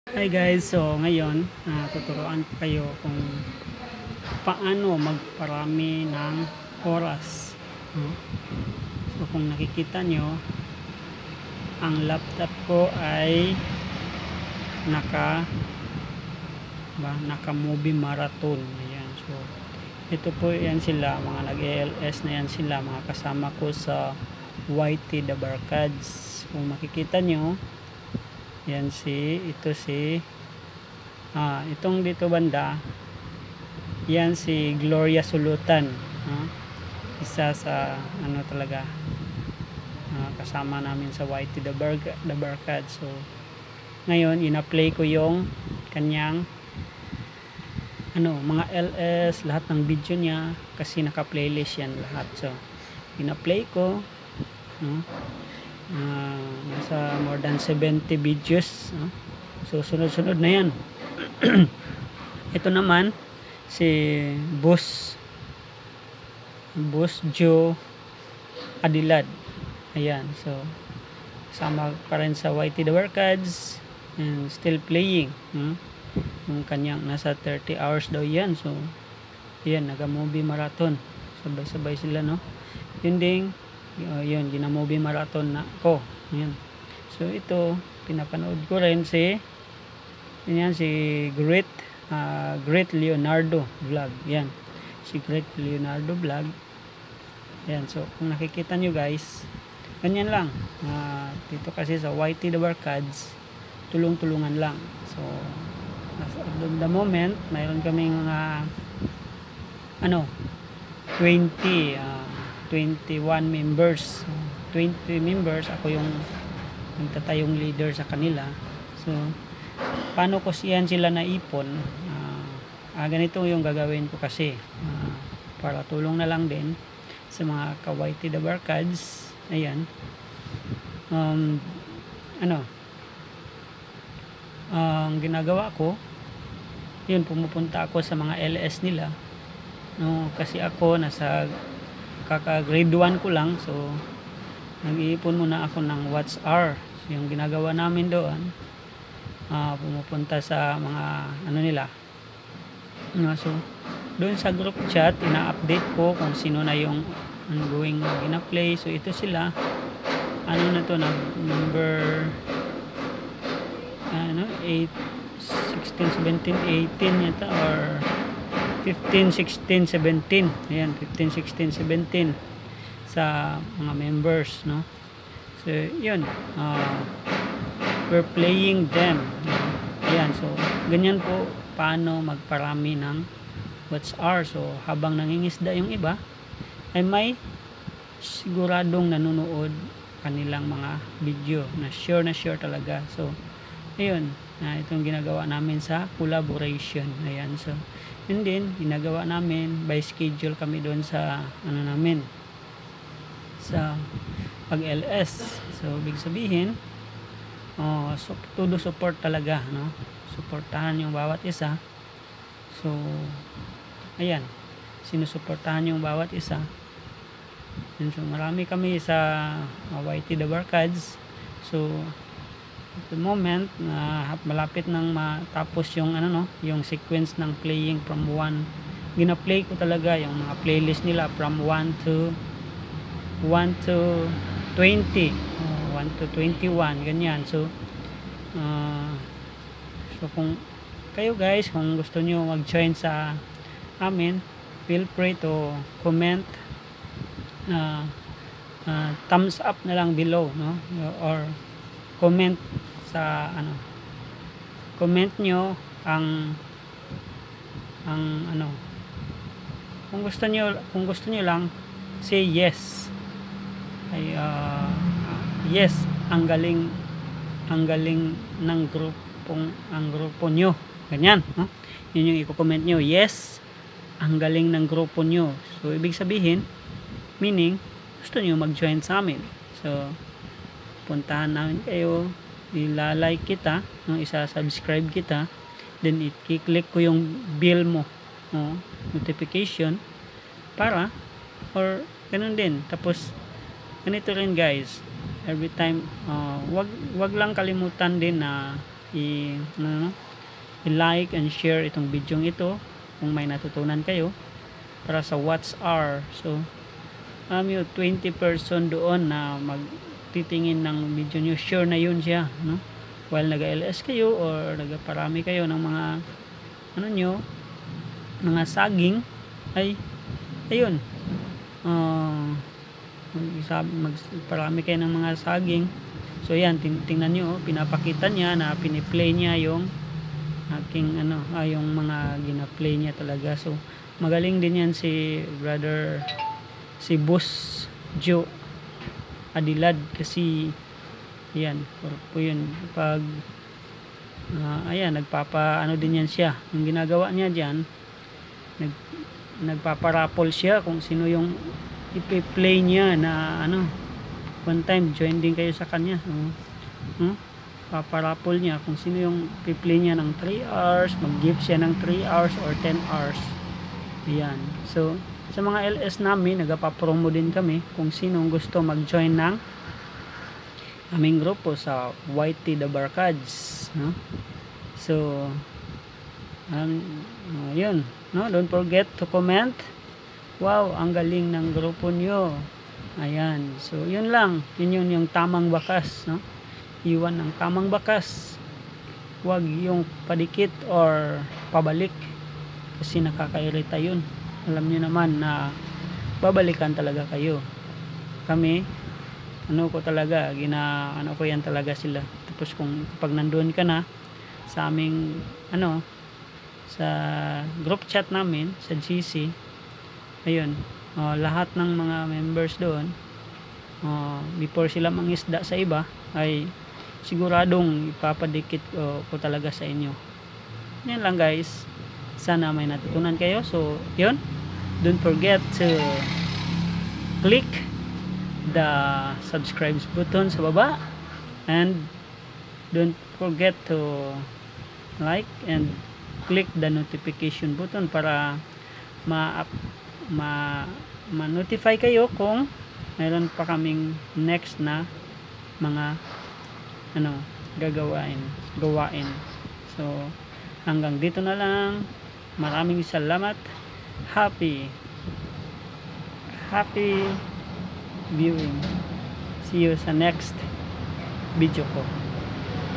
Paano magparami ng Oras at Saging using Facebook Messenger? | Sasagutin yan sa bidyong ito
Paramihan ng kaibigan kasama sina Boss Jhoe Adilad, Gloria Sulotan, Greatleonardo at YT Dabarkads.